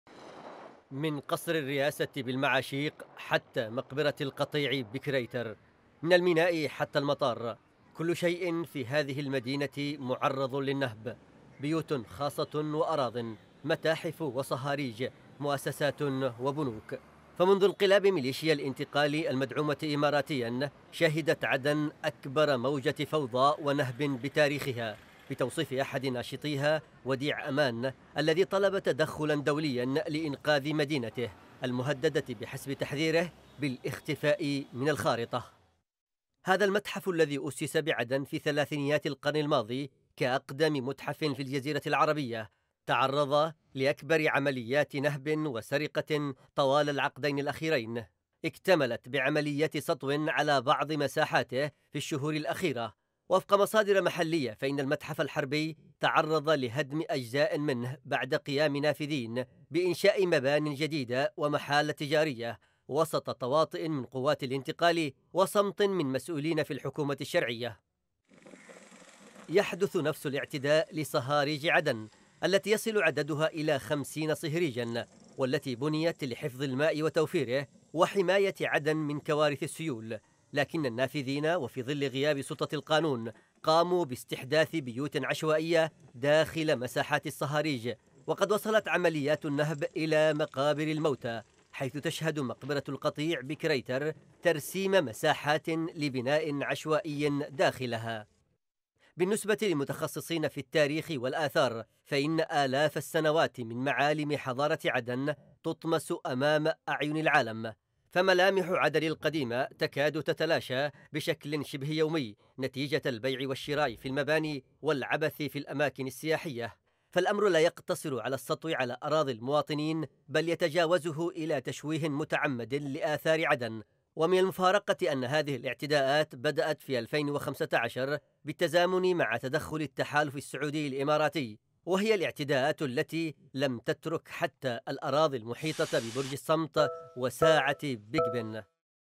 عمليات نهب في عدن طالت المتحف الحربي والصهاريج وعدداً من المؤسسات والأراضي | تقرير: محمد اللطيفي
شهدت عدن منذ 2015 عمليات نهب منظمةً طالت معالم الحضارة في المدينة.
وتزايدت هذه الاعتداءات مع انقلاب مليشيا الانتقالي المدعومة إماراتيا، ودخلت المدينةُ في فوضى من البناء العشوائي بمتحف عدن وصهاريجها والأراضي المحيطة ببرج الصمت وساعة بيج بن، في ظل صمت الحكومة، وتحت أعين قوات التحالف السعودي الإماراتي، ما دفع ناشطين للمطالبة بتدخل دولي لحماية عدن وإنقاذها.